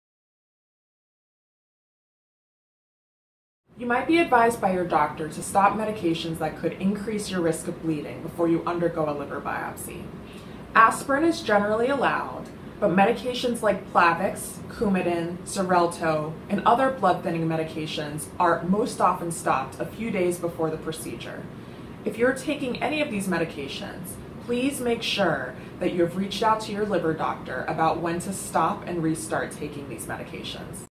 What do I need to do to prepare for a liver biopsy?
Dr. Nneke Ufere from the Massachusetts General Hospital Liver Center discusses how to prepare for a liver biopsy and what to expect during and after the procedure.